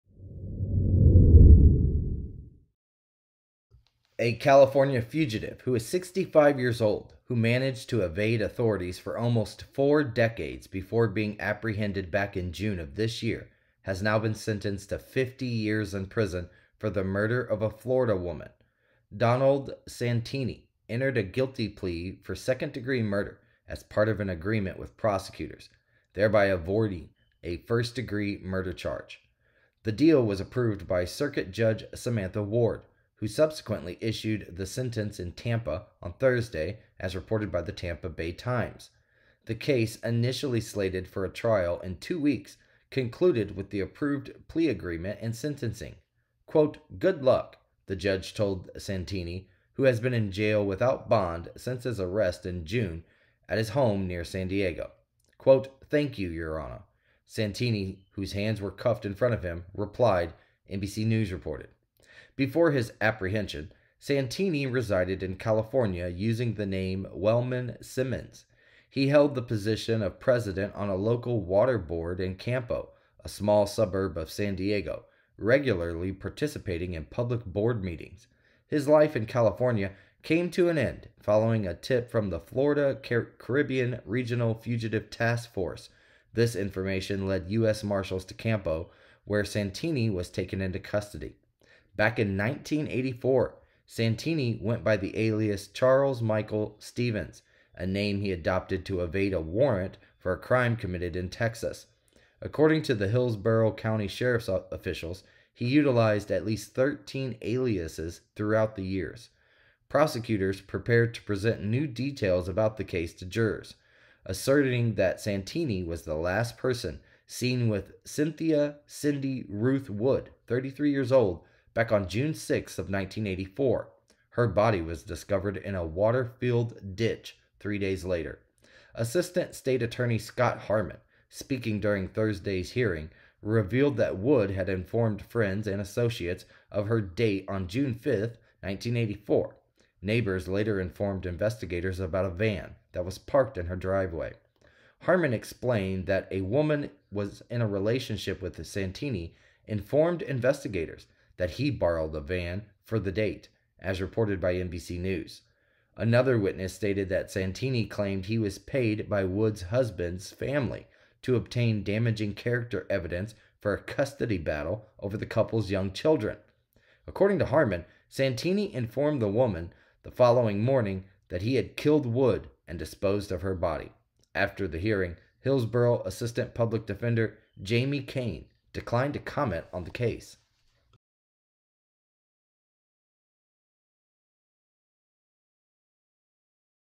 Man who eluded authorities for nearly four decades in woman’s murder sentenced to 50 years
A California fugitive, aged 65, who managed to evade authorities for almost four decades before being apprehended in June, has now been sentenced to 50 years in prison for the murder of a Florida woman.

Donald Santini entered a guilty plea for second-degree murder as part of an agreement with prosecutors, thereby avoiding a first-degree murder charge.

The deal was approved by Circuit Judge Samantha Ward, who subsequently issued the sentence in Tampa on Thursday, as reported by the Tampa Bay Times.

The case, initially slated for a trial in two weeks, concluded with the approved plea agreement and sentencing.

“Good luck,” the judge told Santini, who has been in jail without bond since his arrest in June at his home near San Diego.

“Thank you, Your Honor,” Santini, whose hands were cuffed in front of him, replied, NBC News reported.